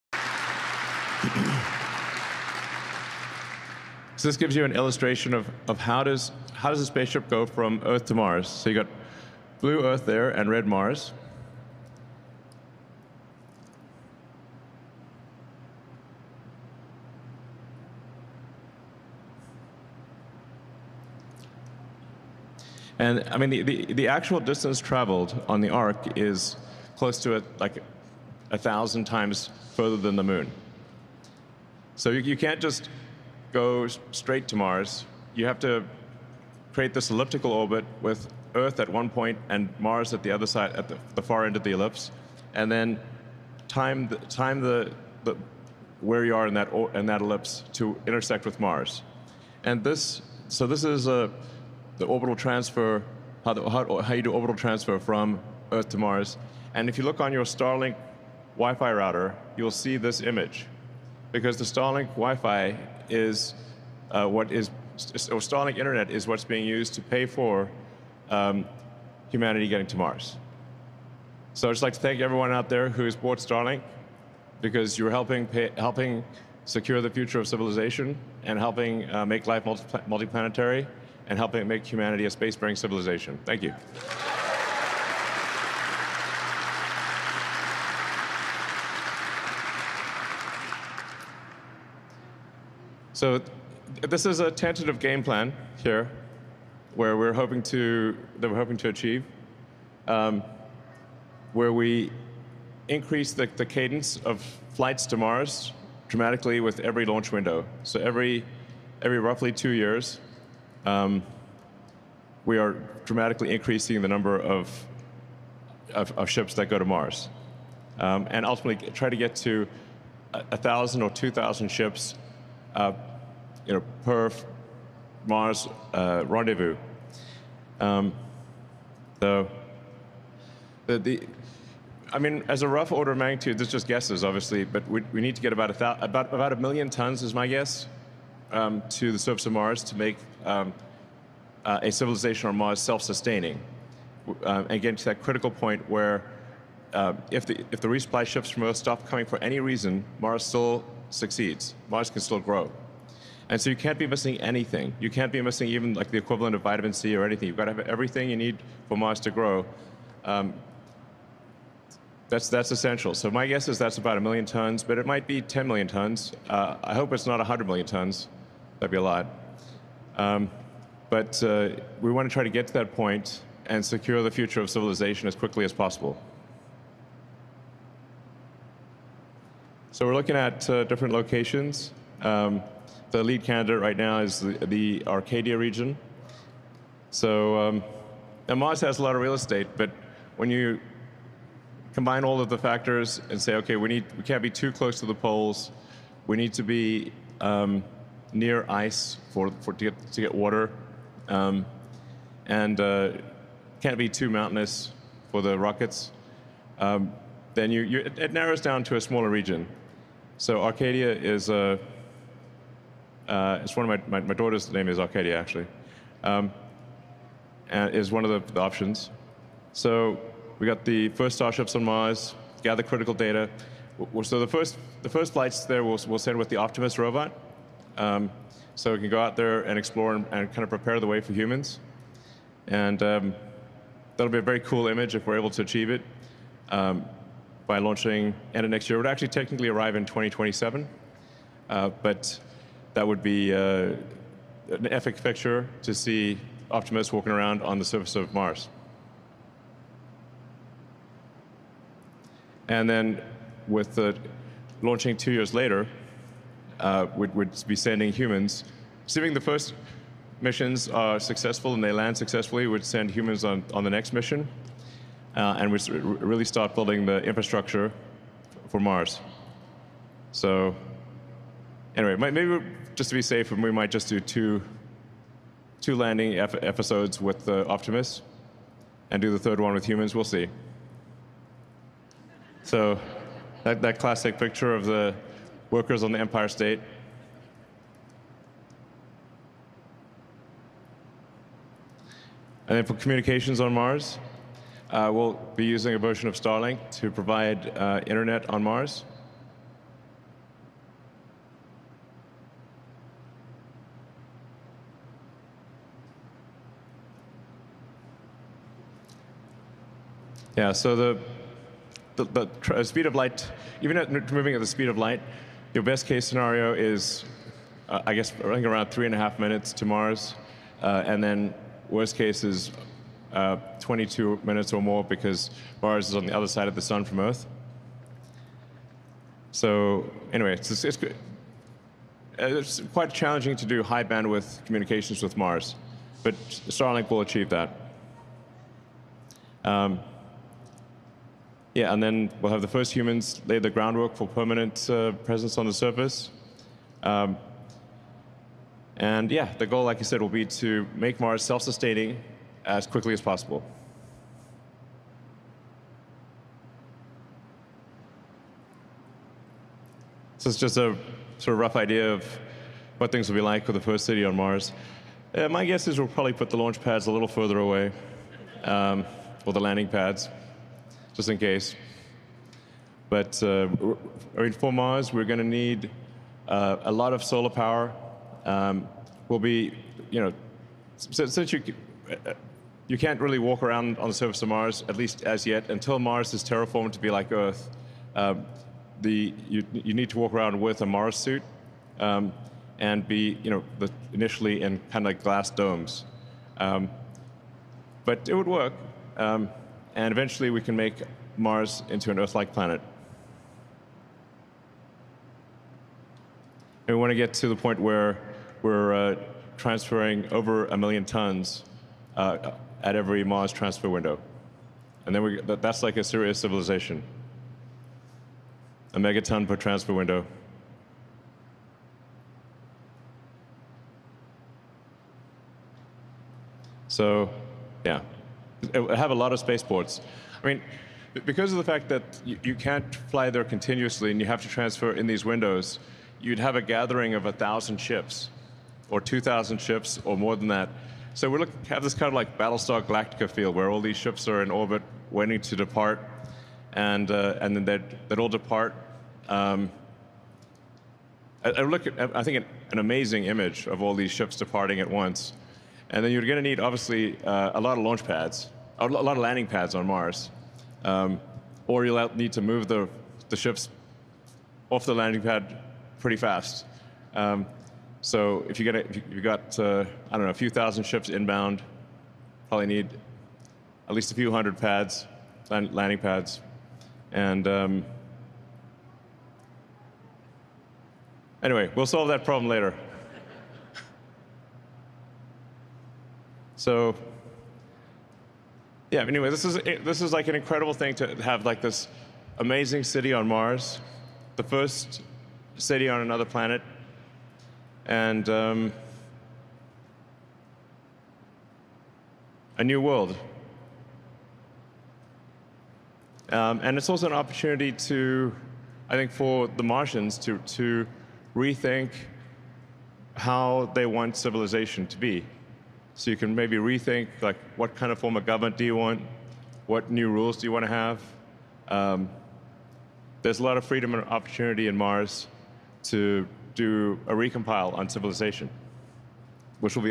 Elon Musk shares 'tentative' Mars game plan in SpaceX update at Starbase
Elon Musk talks about SpaceX's ambitious Mars goals in a company update at Starbase.

Credit: SpaceX